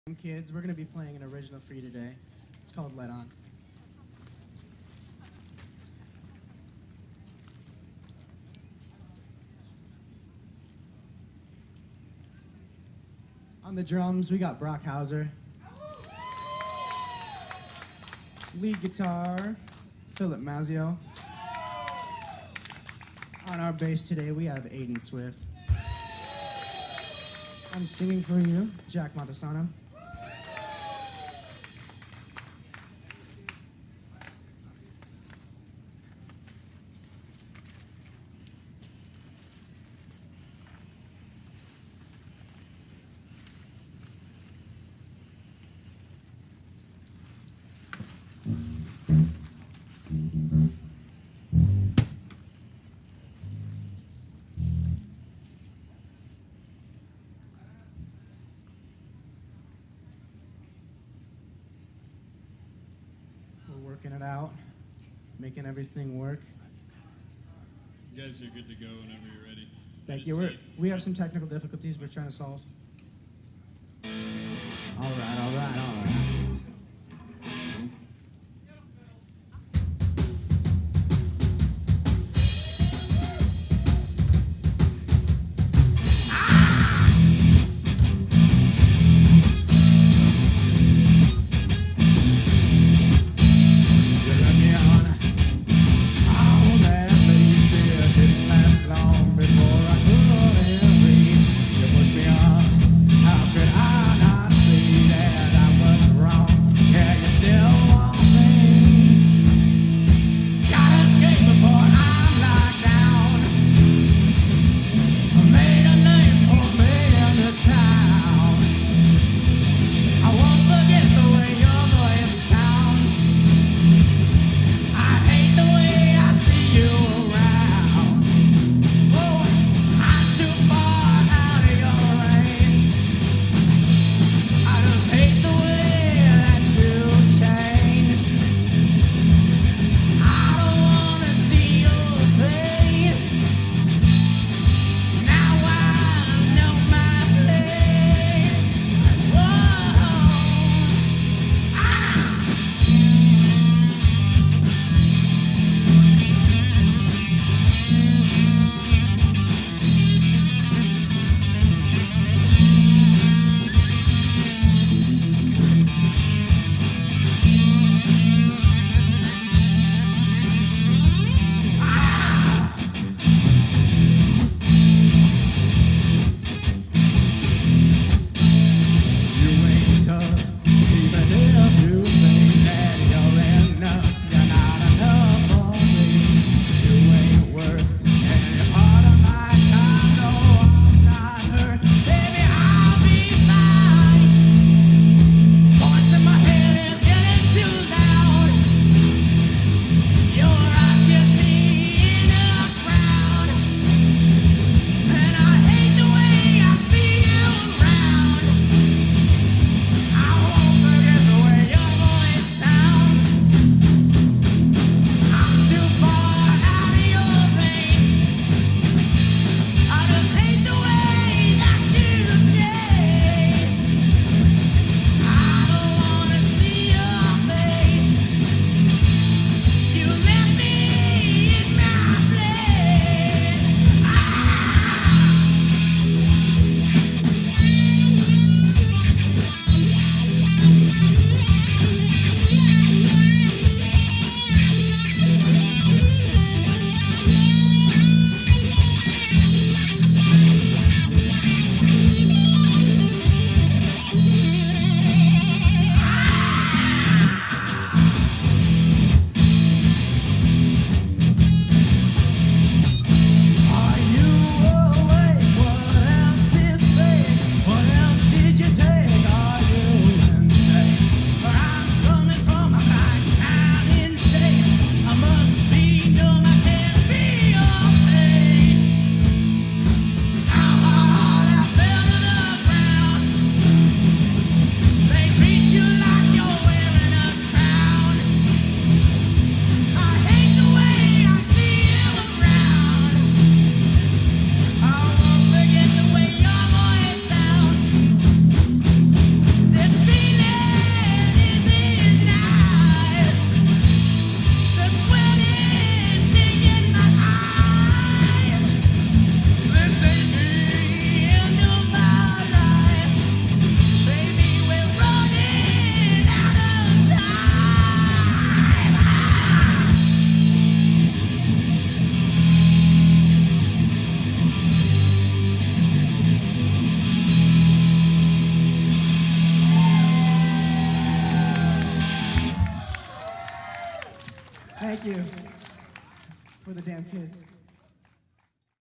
The Darn Kids Proof - Auditions (2021)
The 2021 Auditions of Proof Is In The Pudding music competition were held in beautiful Mesa, AZ at The Rock Teen Center Mesa the day after the exciting grand opening.

Produced by Alice Cooper's Solid Rock Teen Centers, Proof Is In The Pudding is one of the most well known music competitions in the U.S. Musicians ages 12-26 from ALL music genres compete to win the opportunity to open for Alice Cooper and other celebrities at the annual Alice Cooper's Christmas Pudding fundraiser! Plus, a band can win $1,000.00 cash and a solo artist can win $500.00 along with a FREE Taylor Guitar and many other prizes!

Alice Cooper's Solid Rock Teen Centers offers FREE classes for teens in music, art, dance, photography, and more!